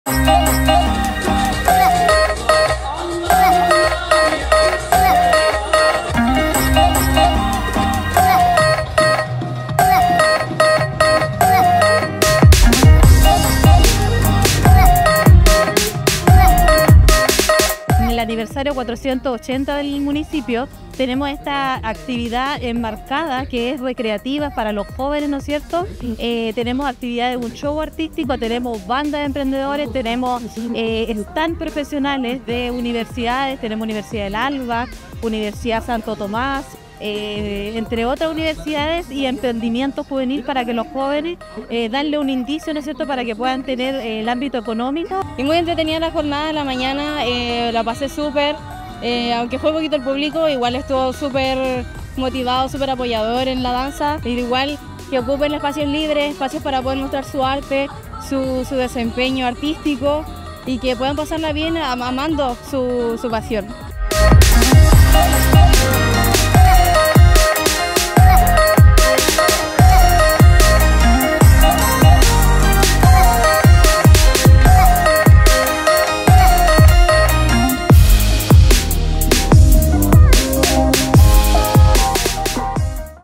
Día Internacional de la Juventud
Un entretenido evento organizado
por la Sección Infancia y Juventud del municipio en la Plaza de Armas.